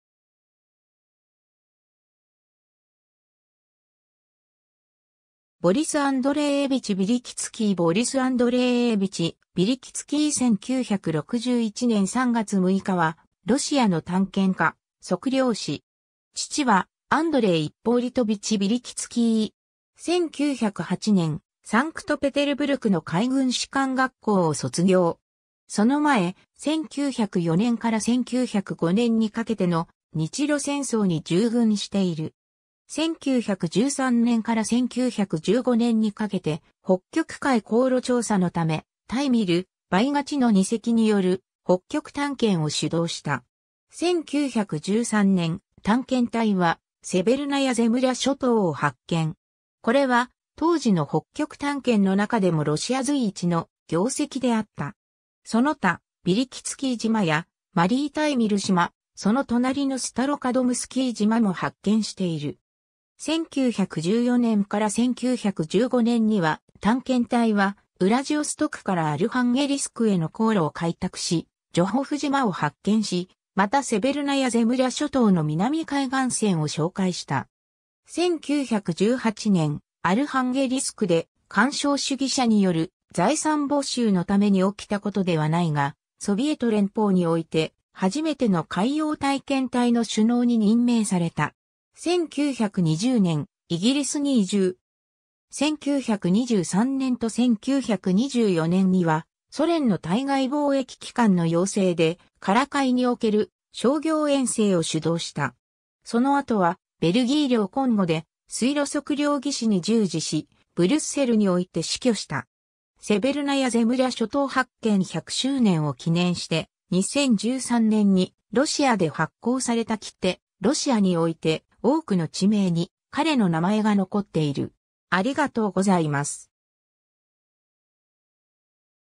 ボリス・ヴィリキツキー
ボリス・アンドレーエヴィチ・ヴィリキツキー ボリス・アンドレーエヴィチ・ヴィリキツキー（ロシア語: Борис Андреевич Вилькицкий（1885年3月22日（新暦4月3日） - 1961年3月6日））は、ロシアの探検家、測量師。
父はアンドレイ・イッポリトヴィチ・ヴィリキツキー。
 1908年、サンクトペテルブルクの海軍士官学校を卒業。
その前、1904年から1905年にかけての日露戦争に従軍している。
1913年から1915年にかけて、北極海航路調査のため「タイミル」「ヴァイガチ」の二隻による北極探検を主導した。
1913年、探検隊はセヴェルナヤ・ゼムリャ諸島を発見。
これは当時の北極探検の中でもロシア随一の業績であった。
その他、ヴィリキツキー島やマリー・タイミル島、その隣のスタロカドムスキー島も発見している。
1914年から1915年には、探検隊はウラジオストクからアルハンゲリスクへの航路を開拓し、ジョホフ島を発見し、またセヴェルナヤ・ゼムリャ諸島の南海岸線を紹介した。
 1918年、アルハンゲリスクで干渉主義者による財産没収のために起きたことではないが、ソビエト連邦において初めての海洋探検隊の首脳に任命された。
1920年、イギリスに移住。
1923年と1924年には、ソ連の対外貿易機関の要請でカラ海における商業遠征を主導した。
 その後は、ベルギー領コンゴで水路測量技師に従事し、ブリュッセルにおいて死去した。
 セヴェルナヤ・ゼムリャ諸島発見100周年を記念して、2013年にロシアで発行された切手 ロシアにおいて多くの地名に、彼の名前が残っている。